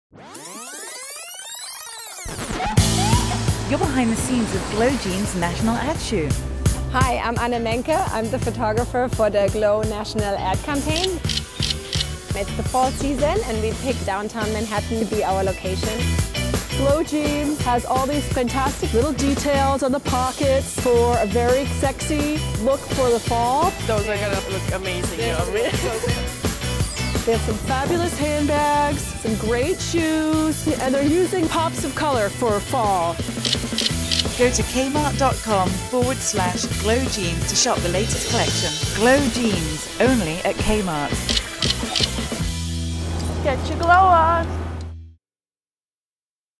Behind the Scenes in Manhattan with Glo Jeans
Go behind the scenes with Anne Menke as she photographs this years' national Glo ad campaign in downtown Manhattan.  Get a sneak peak at the sexy jeans, the fabulous shoes, and the great handbags, all featuring great pops of color for the fall!  Get your Glo on!